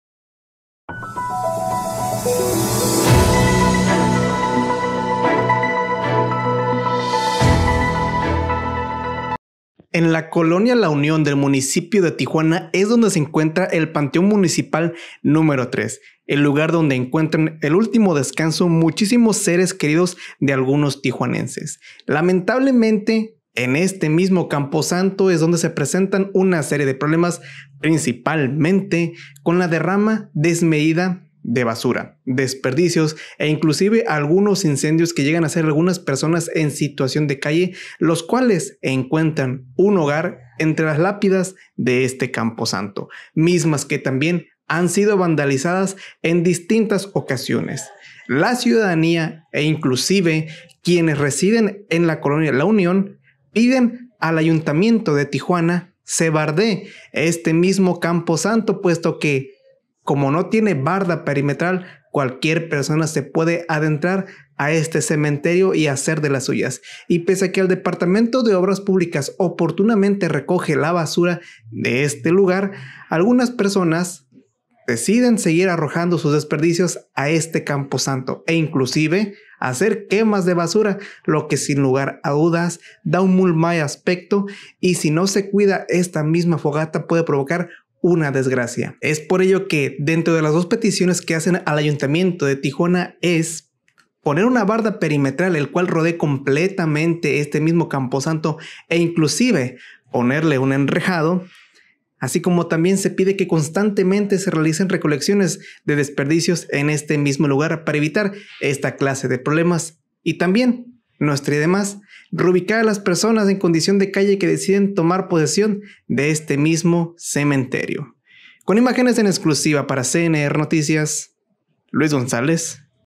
CNR Tijuana De panteón a basurero
En anteriores ocasiones le hemos hecho saber por el abandono que sufren algunos de los espacios públicos de Tijuana, lamentablemente la poca atención por parte de las autoridades a afectado gravemente al panteón municipal #3 ubicado en la colonia la unión del municipio de Tijuana, tan solo llegar al lugar nos podemos percatar de grandes cúmulos de basura, lápidas completamente abandonadas así como personas en situación de calle viviendo en este camposanto, en algunas ocasiones se pueden ver quemas de basura dentro de las instalaciones del cementerio lo que sin lugar a dudas preocupa de sobremanera a las personas que viven en esta demarcación, se pide la rápida intervención de las autoridades municipales para darle pronta solución a estas problemáticas.